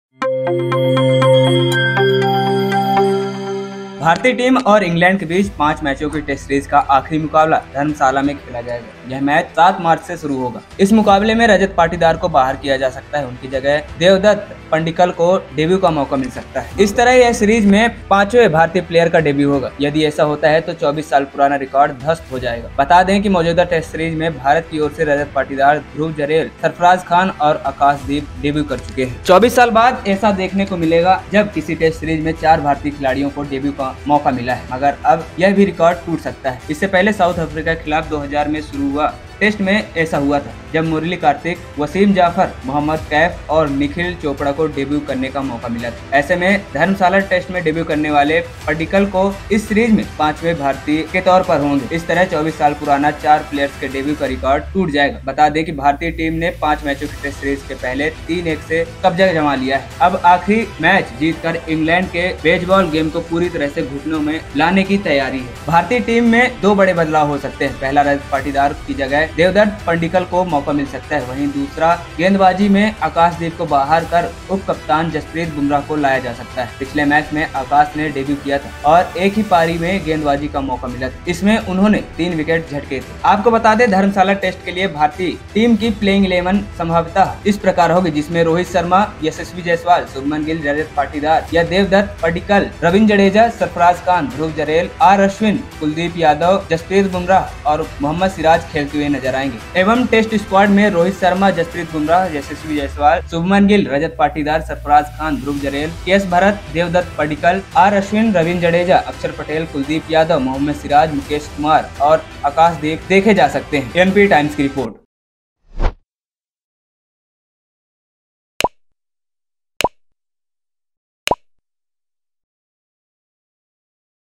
इंग्लैण्ड को घुटने में लाने की तैयारी हुई पूरी, इस खिलाडी को डेब्यू करने को मिलेगा मौका
इस ख़ास रिपोर्ट में देखिये इंग्लैण्ड को घुटने में लाने की तैयारी हुई पूरी, इस खिलाडी को डेब्यू करने को मिलेगा मौका

ANPTIMES
anptimes